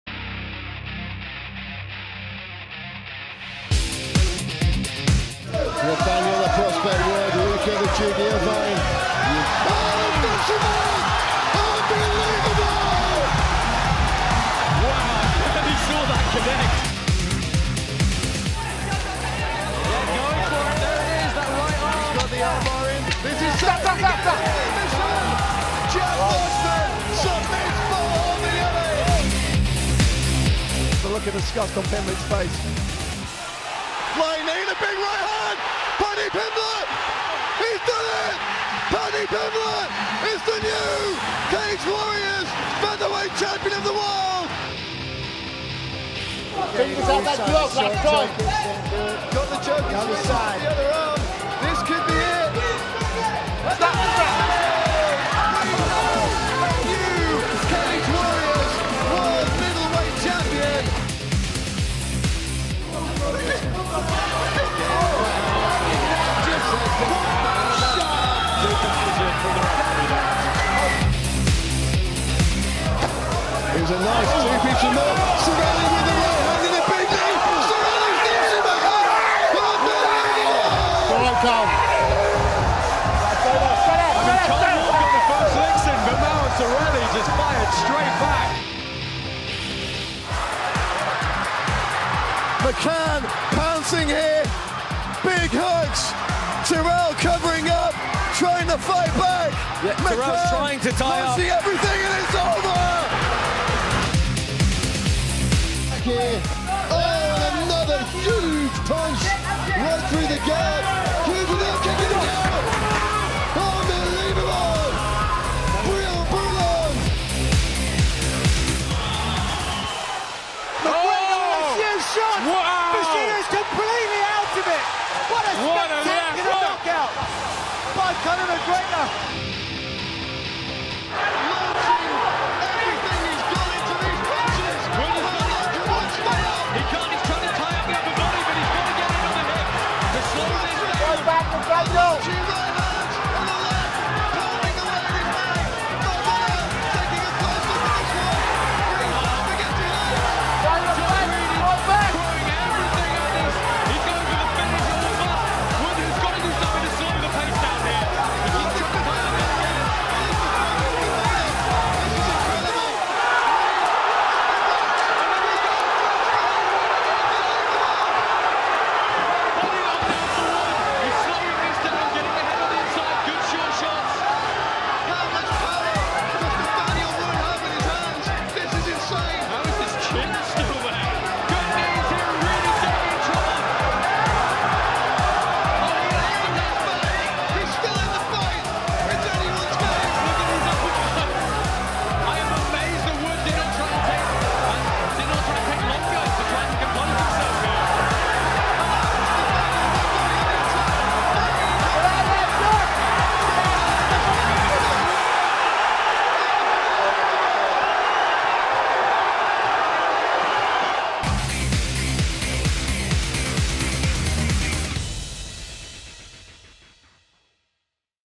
Top 10 insane wins in Cage Warriors history
Knockouts, submissions and some of the craziest fights on record. Here's the top 10 wins in Cage Warriors history, as chosen by Eleven Sports Network, featuring Conor McGregor, Joanna Jedrzejczyk and more.